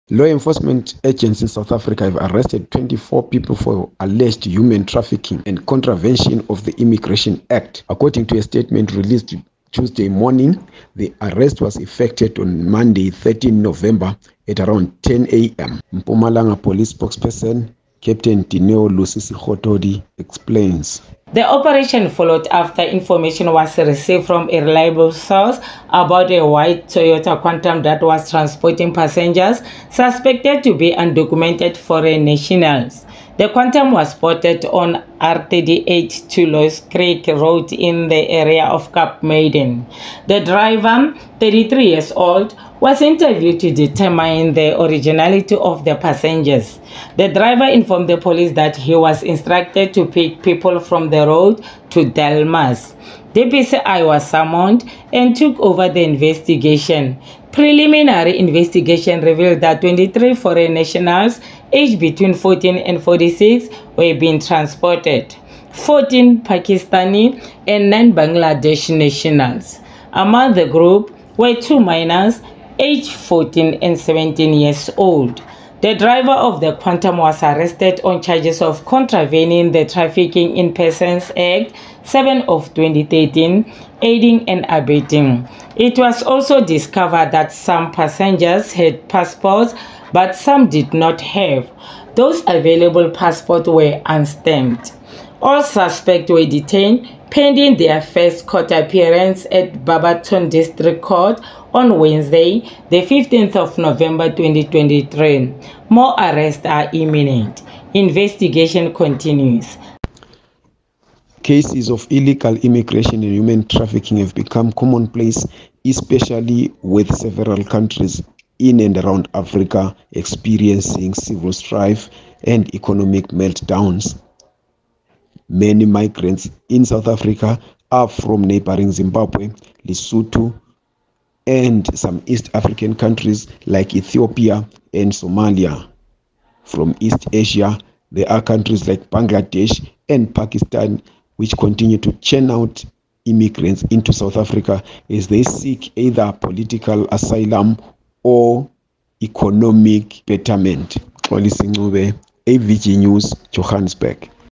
24 ARRESTED AND COUNTING: South Africa gets tough on illegal migration, human trafficking
South Africa gets tough on illegal migration, human trafficking